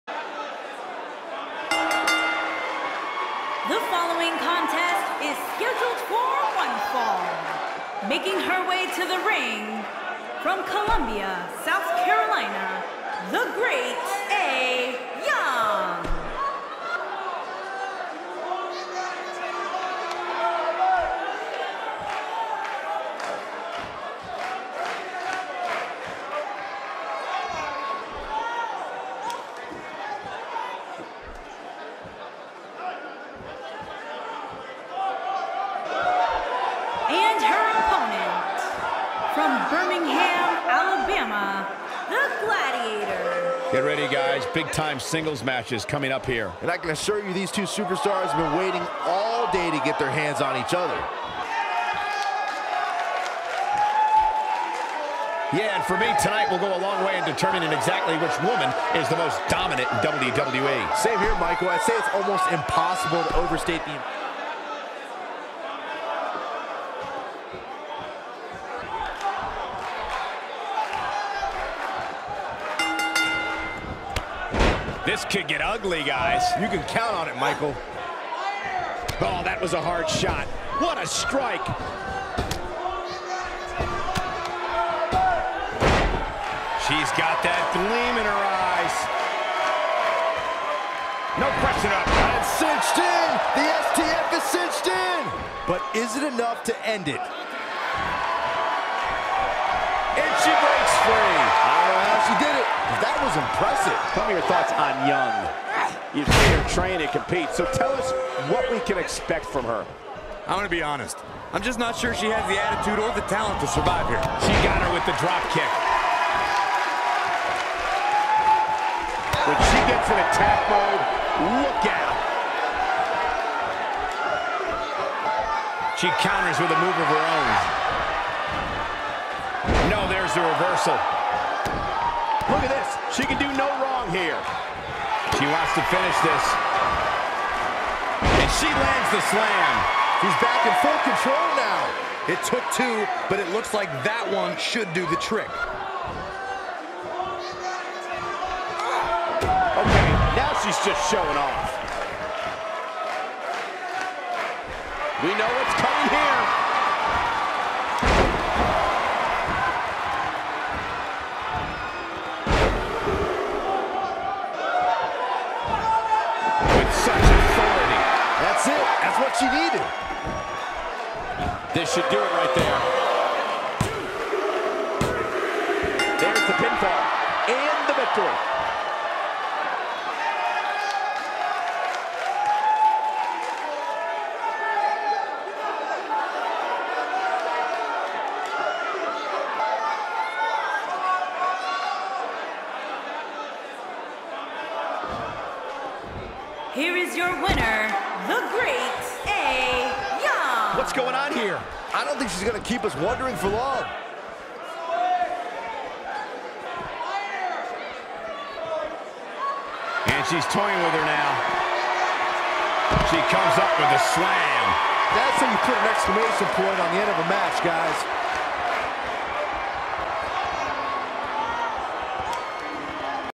SHOOT Match: The Great Mae Young VS Gladys Kill'em Gillem
July 17, 1941 in Columbus, Ohio
The Great Mae Young takes on Gladys Kill'em Gillem in a shoot match. WWE Hall of Famer, Mae Young was truly a rough gal and truly proved that WOMEN in wrestling were just as tough (or tougher) than the men.